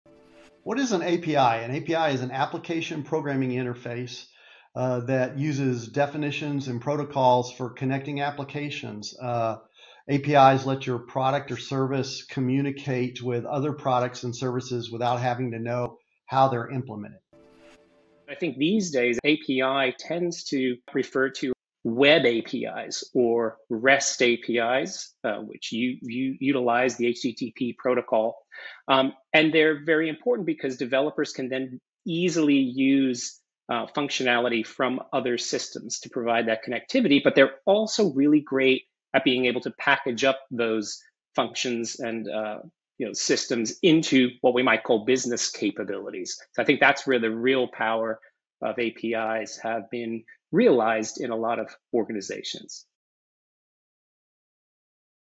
What is an API? Axway experts answer API questions.
An API is an Application Programming Interface that uses definitions and protocols for connecting applications. APIs let your product or service communicate with other products and services without having to know how they're implemented.

These days, API tends to refer to web APIs or REST APIs, which utilize the HTTP protocol, and they're very important because developers can then easily use functionality from other systems to provide that connectivity.

But they're also great at being able to package up those functions and systems into what we might call business capabilities. That's where the real power of APIs has been realized in a lot of organizations.

Visit @AxwaySoftware for more!